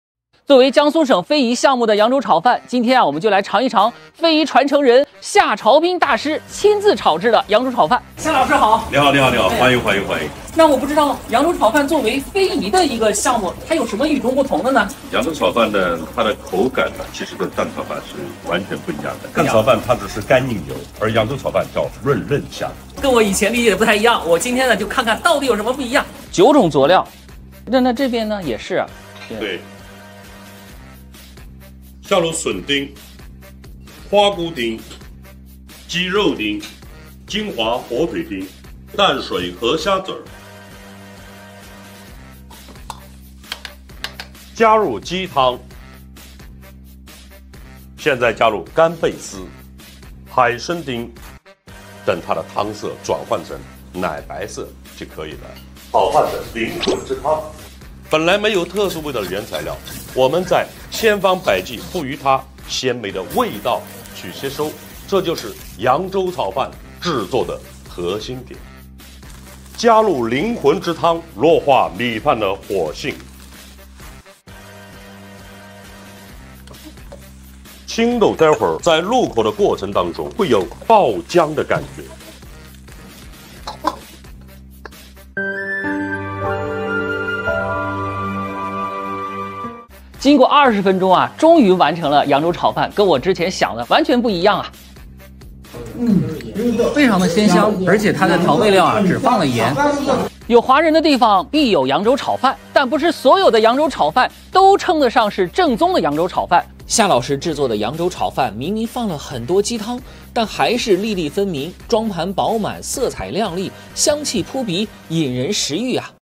非遗扬州炒饭，究竟是啥样？味道如何呢？非遗
扬州炒饭，究竟是啥样？味道如何呢？

 炒饭,人文,民俗,文化,扬州,味道,非遗